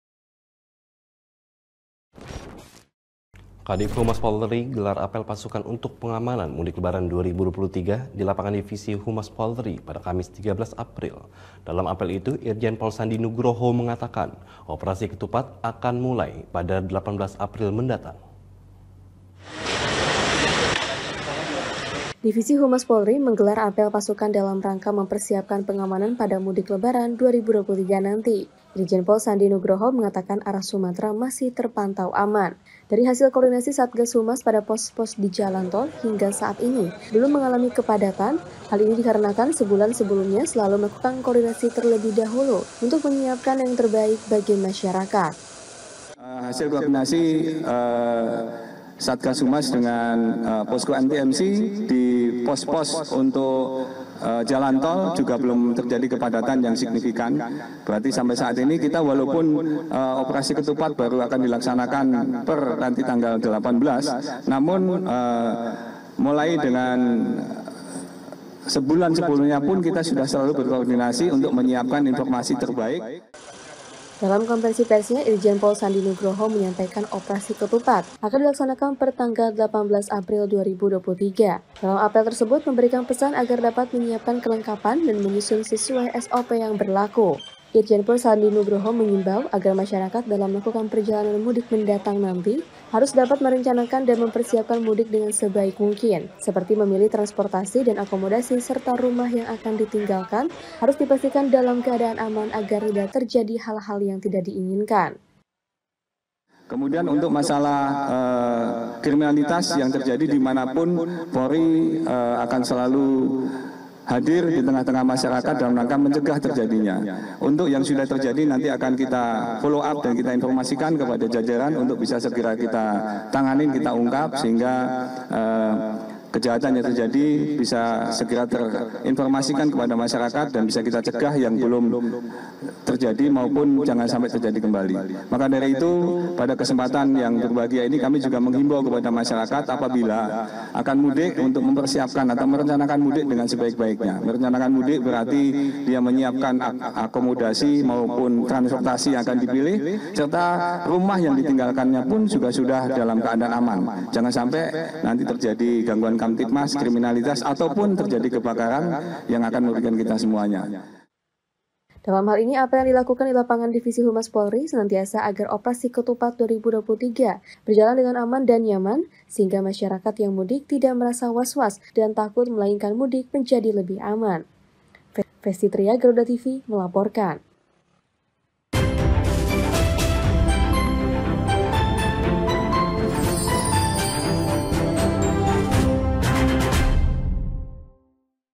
Kadiv Humas Polri Gelar Apel Pasukan Mudik Lebaran 2023
Kadiv Humas Polri gelar apel pasukan untuk pengamanan mudik lebaran 2023 di lApangan Divisi Humas Polri pada Kamis 13 April. Dalam apel itu Irjen Pol Sandi Nugroho mengatakan opeasi ketupat akan dimulai pada 18 April mendatang.

Irjen Pol Sandi Nugroho mengimbau, agar masyarakat dalam melakukan perjalanan mudik mendatang harus dapat merencanakan dan mempersiapkan mudik dengan sebaik mungkin, seperti memilih transportasi dan akomodasi serta rumah yang akan ditinggalkan harus di pastikan dalam keadaan aman agar tidak terjadi hal-hal yang tidak diinginkan.